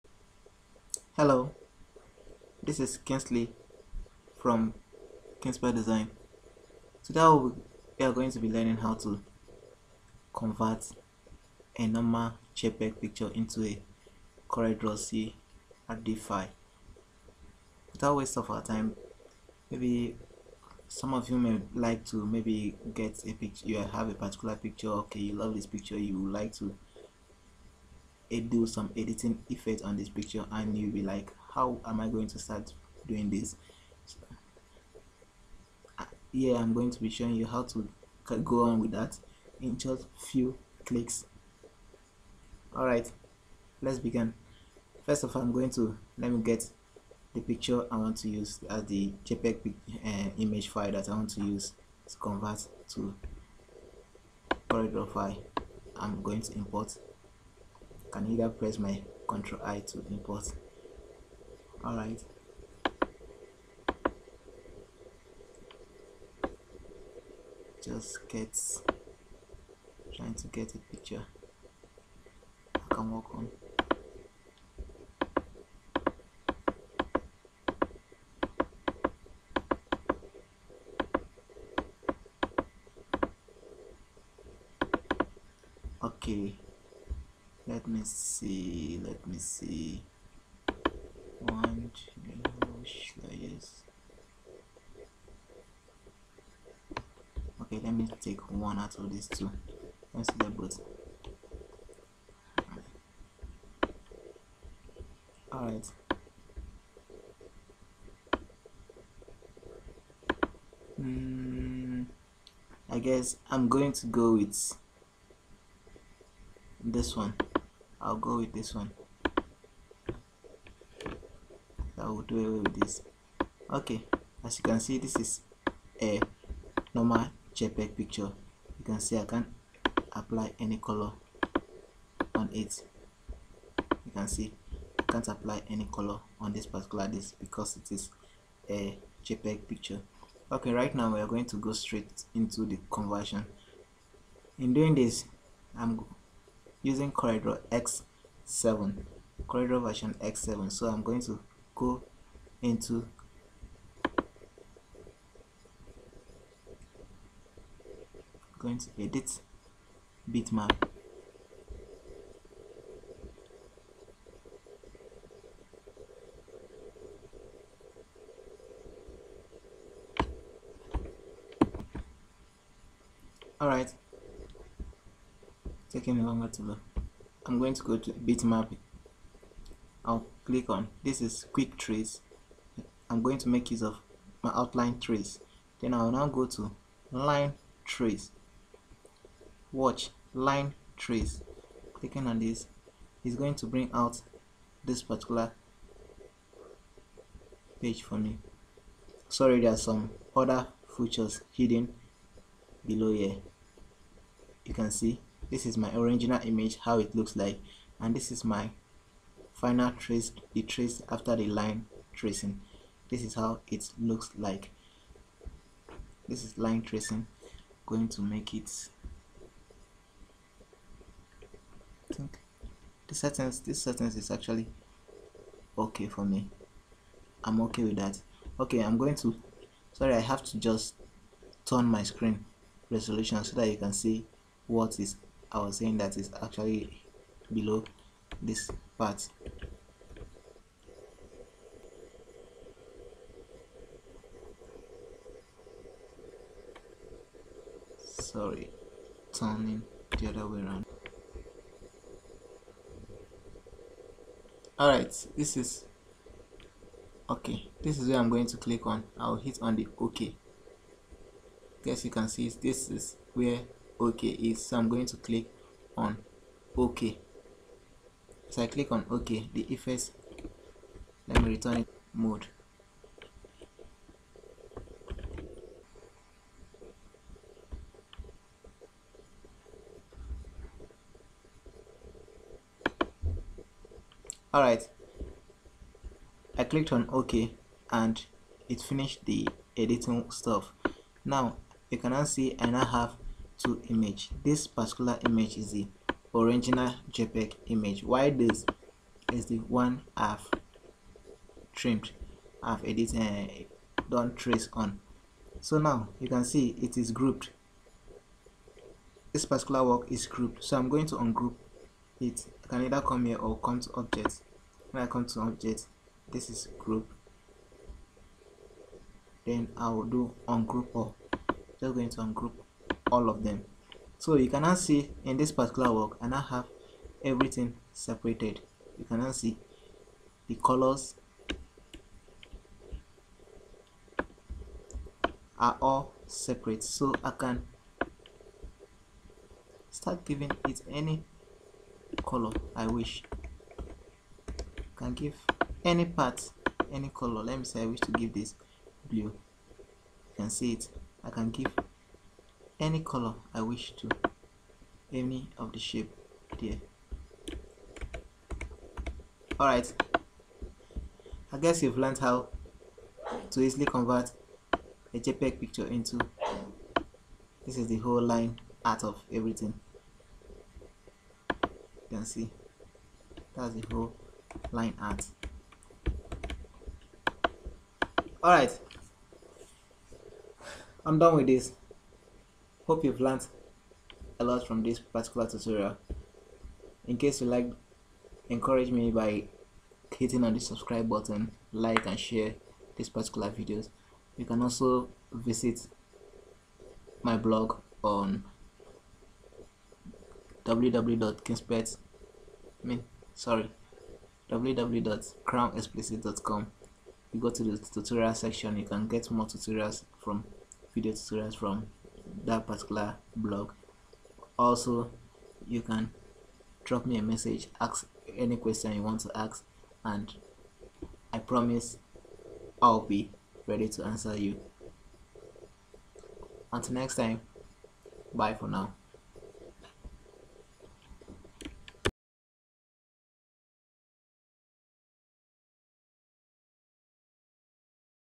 Convert JPEG to CDR IN FEW CLICKS AND SECONDS
This Tutorials shows us how to convert JPG to CDR in Few Clicks.

You can Visit the tutorial section of our blog and get more tutorials.